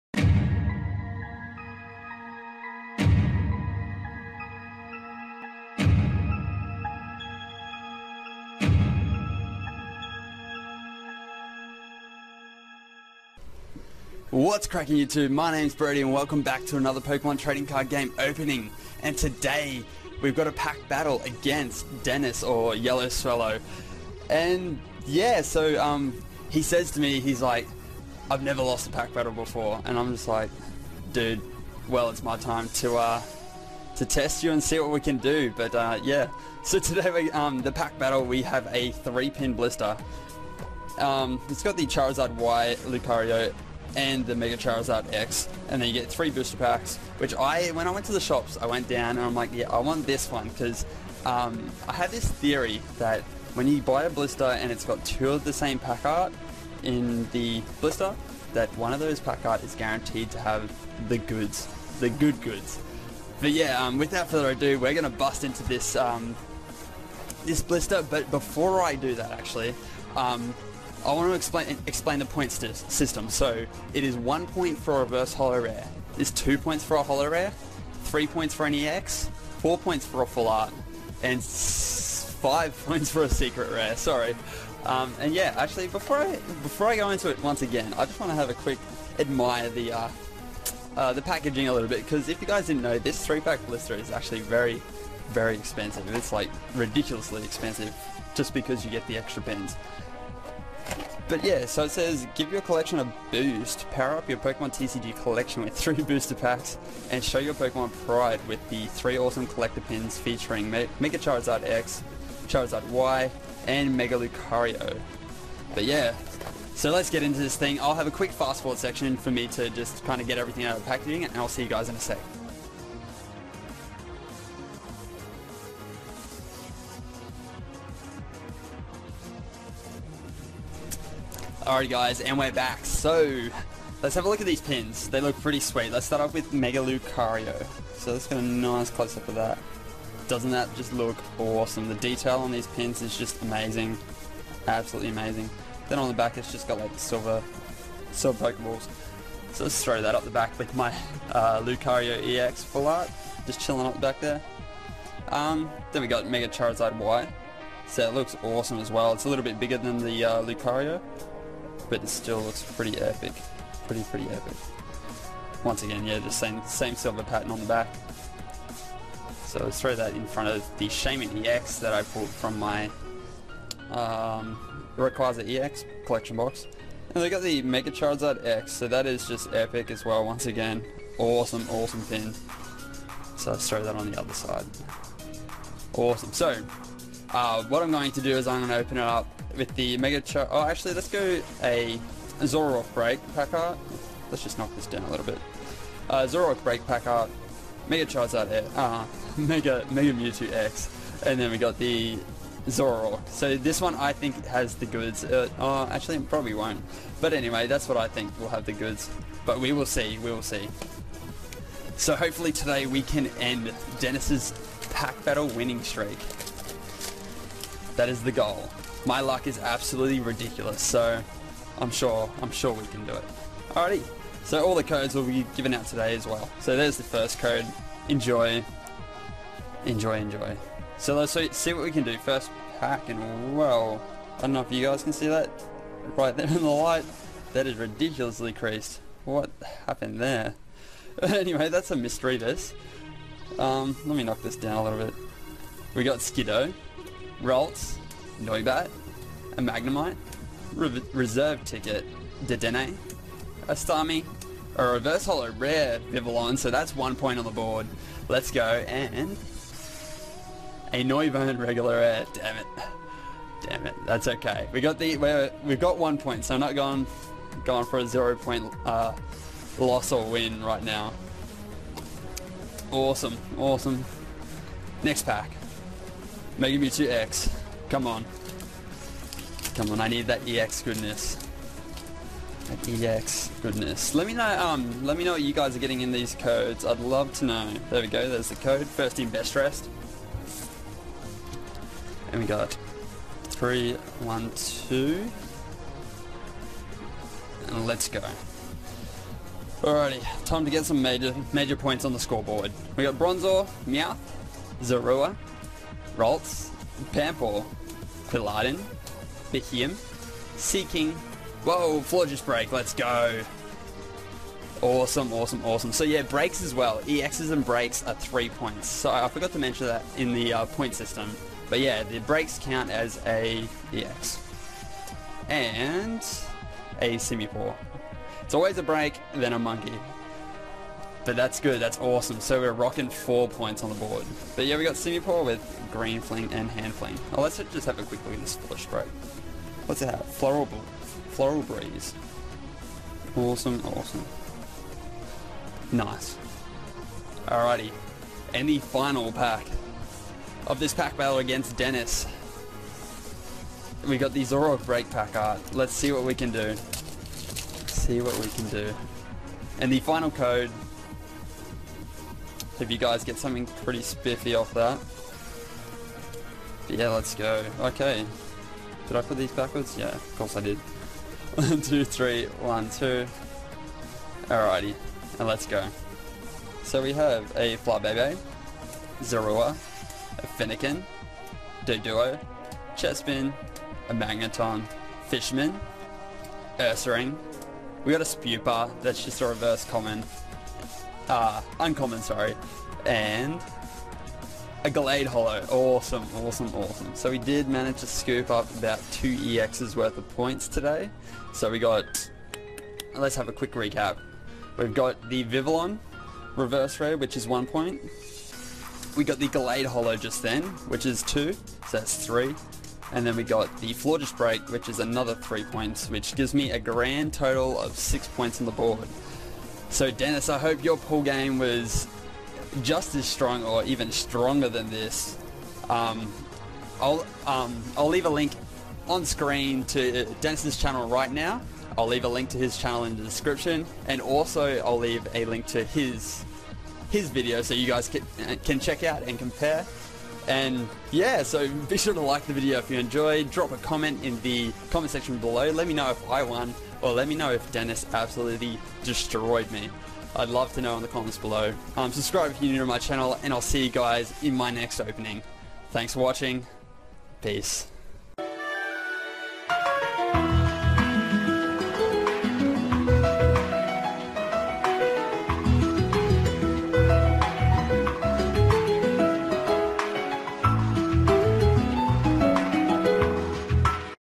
Brodie-Amity TCG vs YellowSwellow - Pokemon TCG Pack Battle
What's Crackin' YouTube! Today we bring out the competitive side with a pack battle against Dennis aka. YellowSwellow!

ENJOY!